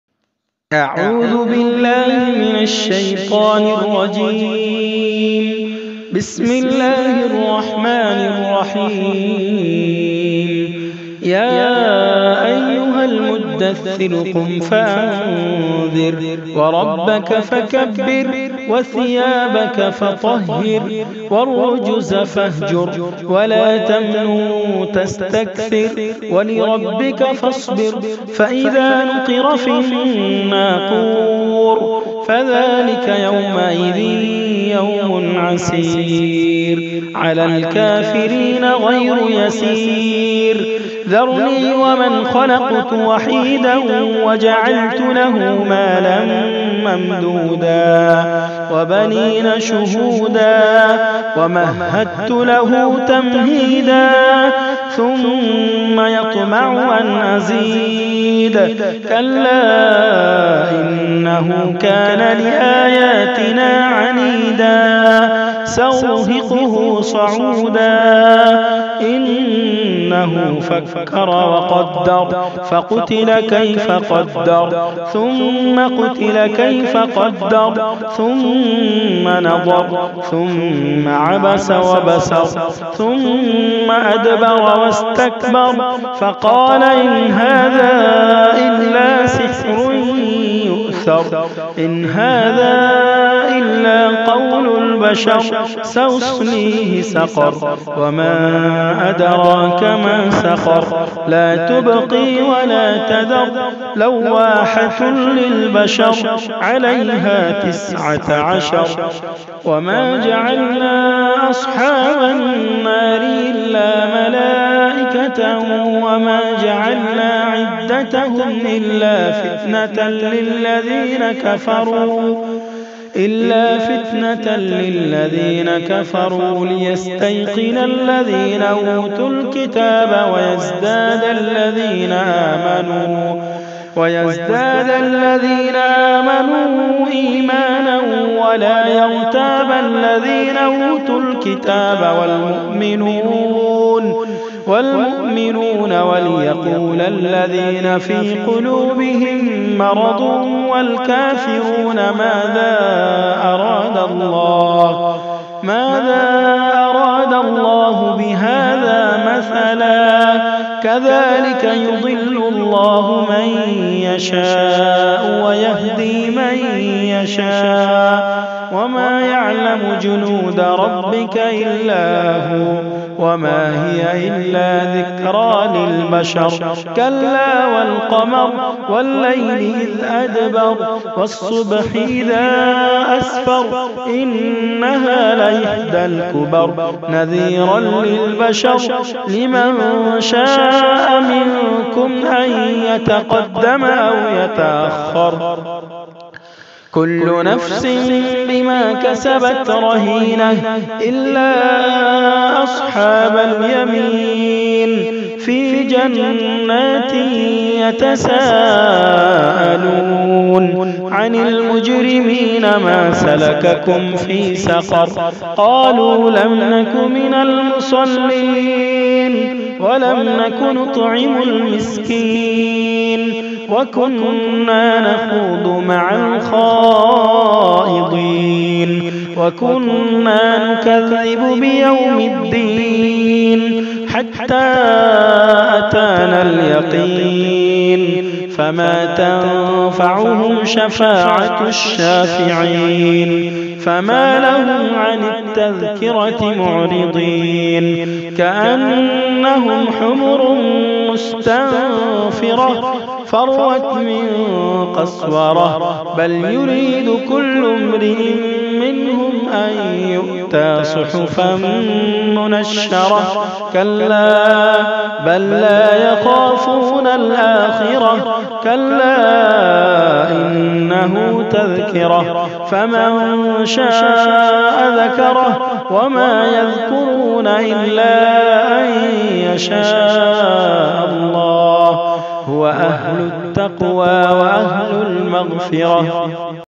Surah Al-Muddassir | Quran Recitation With Arabic Text | surah muddaththir (سورةالمدثر)
🌟presenting tilawat e Quran of Surah full text recited by Qari sadiqul Islam with his beautiful Voice
سورةالمعارج بصوت_جميل
سوره النوح
سوره المعارج